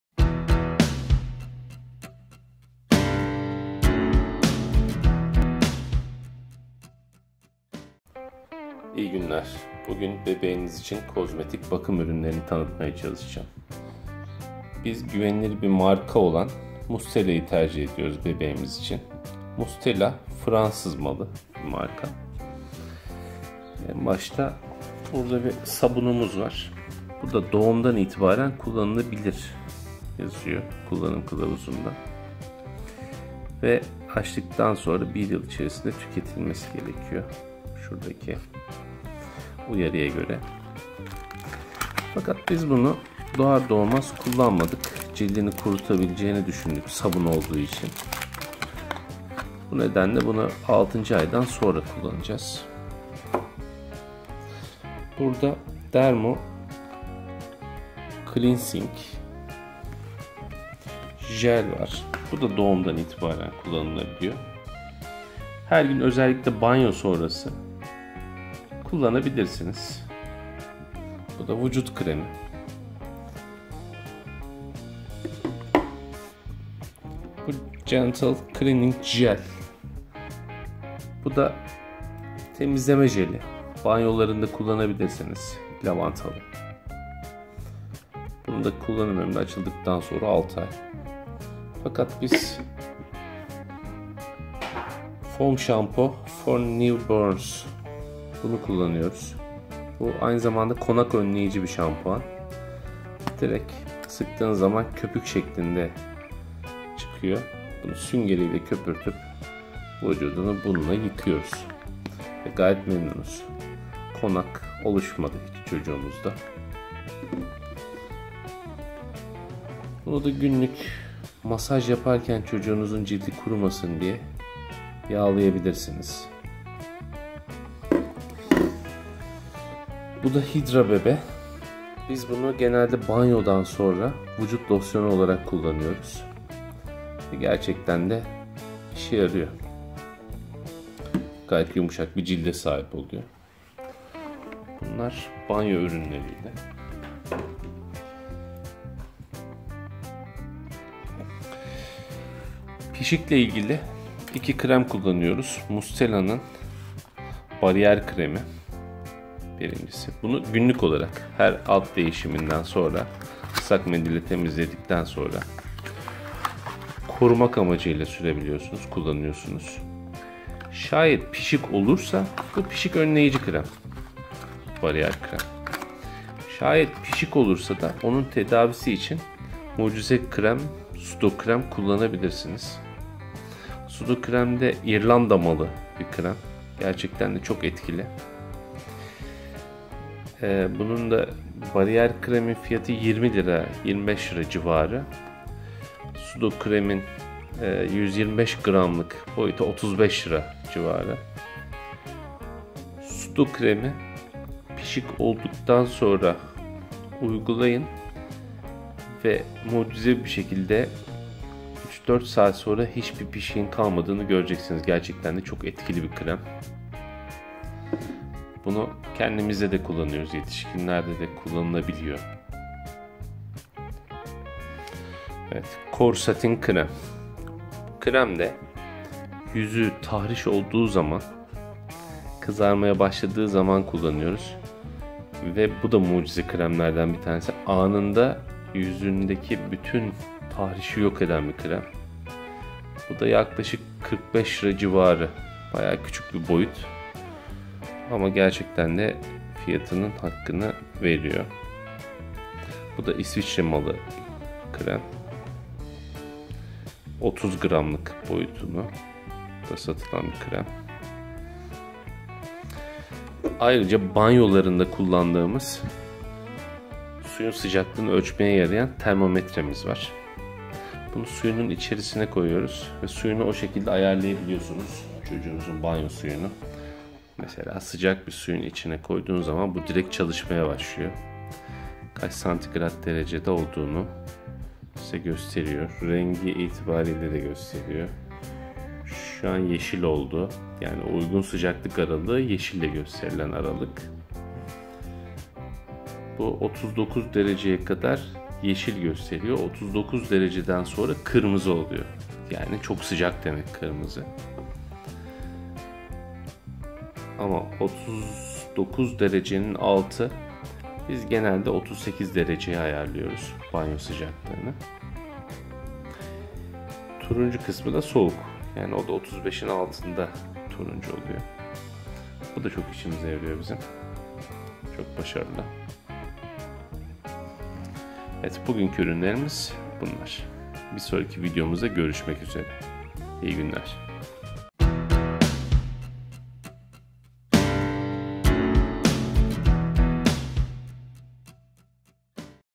EN GÜVENİLİR BEBEK ŞAMPUAN VE KREMLERİ (PİŞİK KREMİ) MUSTELA SUDOCREAM CORESATİN (baby shampoo)
BEBEĞİNİZ İÇİN KULLANABİLECEĞİNİZ ŞAMPUAN, YAĞ, LOSYON, PİŞİK KREMLERİNİ SİZLER İÇİN İNCELEDİK KULLANDIĞIMIZ ÜRÜNLERİ SİZLER İÇİN TANITMAYA ÇALIŞTIK.